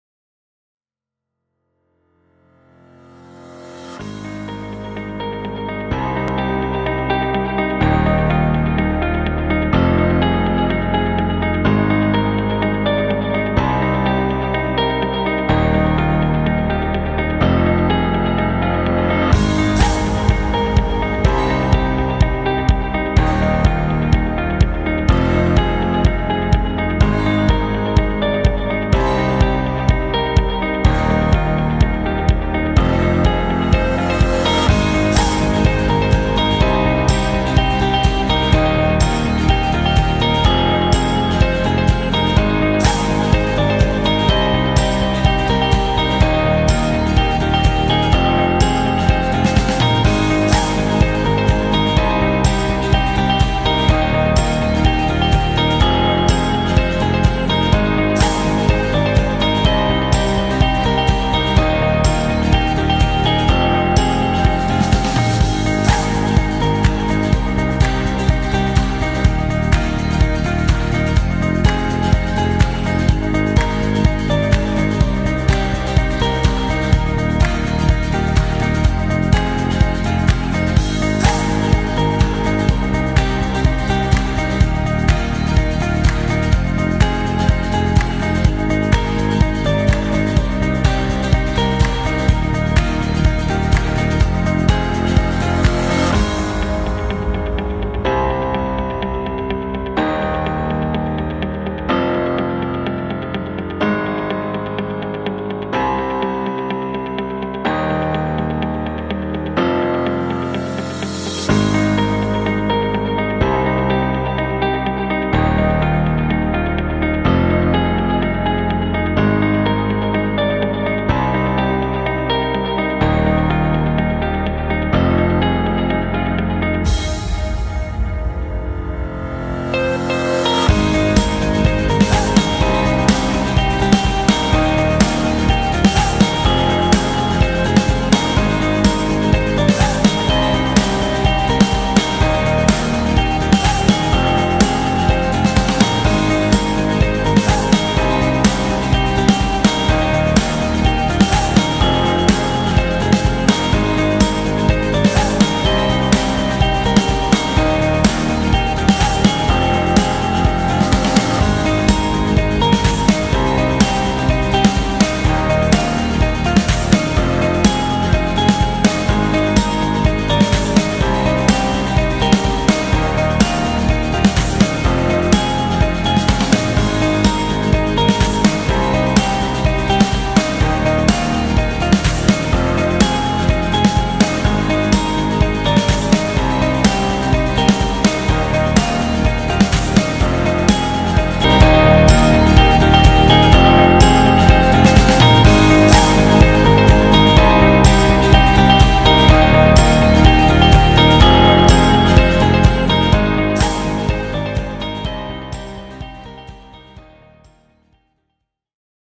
F31 - F32 - F33 Undervoltage Releases Accessory Installation Video
Undervoltage Releases: Used for tripping the circuit breaker when the energy is cut off or voltage goes below 70% the operating voltage. When no energy is supplied to the undervoltage coil, the circuit breaker cannot get back to open position.